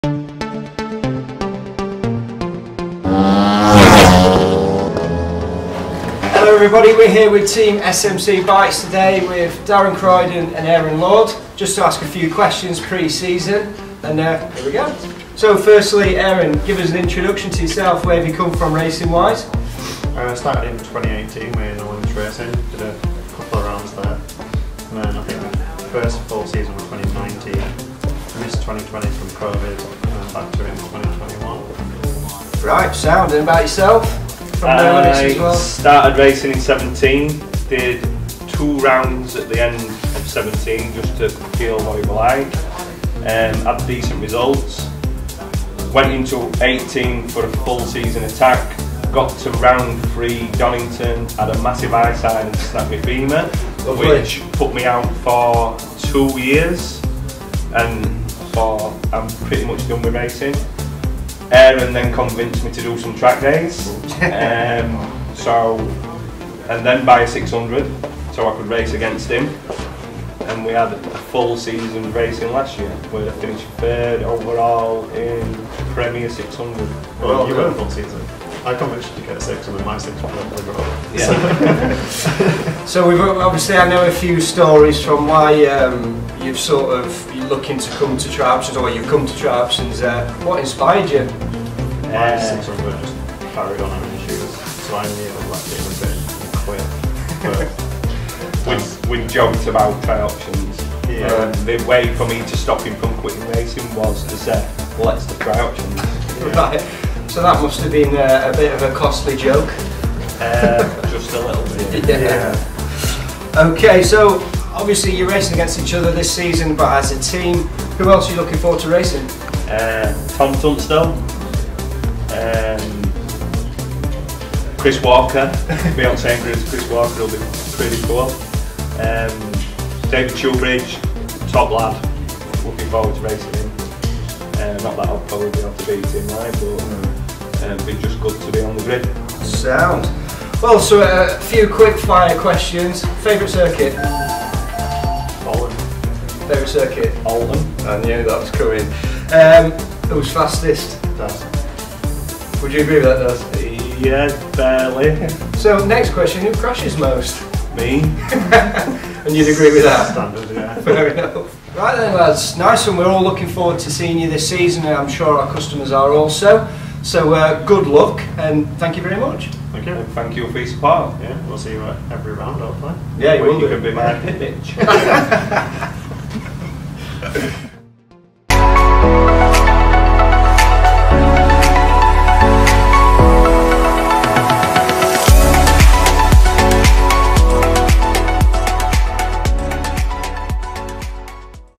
SMC Bikes Racing - Meet the Team - Ducati Tri-Options Cup
Aaron Lord and Darren Croydon will be competing in the Ducati Tri-Options Cup this season onboard their Ducati Panigale V2s.
Sheffield Motorcycle Centre will be supporting their progress throughout the series.

Round 1 - Silverstone
Round 2 - Donnington Park
Round 3 - Donnington Park (World Superbikes)
Round 4 - Brands Hatch GP
Round 5 - Cadwell Park
Round 6 - Snetterton
Round 7 - Oulton Park
Round 8 - Brands Hatch GP